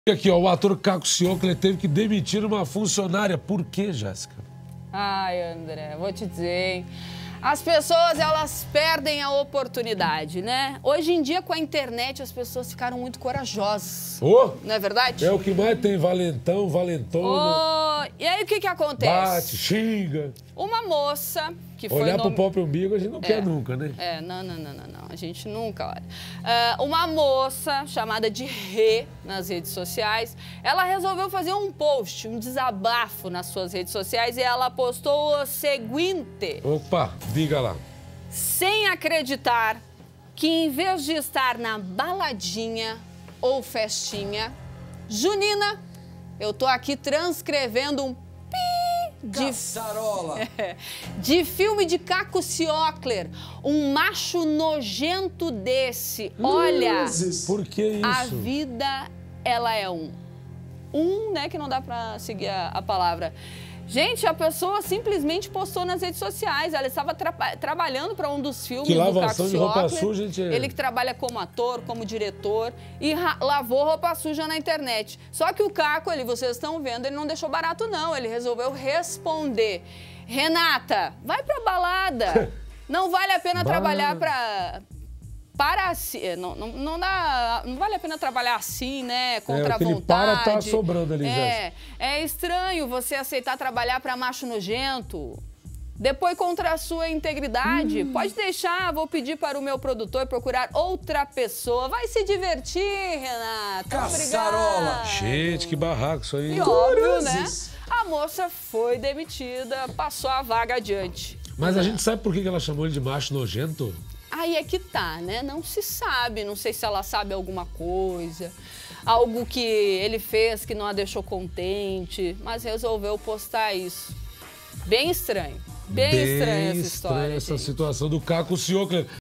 Caco Ciocler demite funcionária após postagem em rede social
O ator viu uma postagem da mulher o chamando de "macho nojento" e decidiu demiti-la. Para assistir ao conteúdo completo, acesse o PlayPlus.com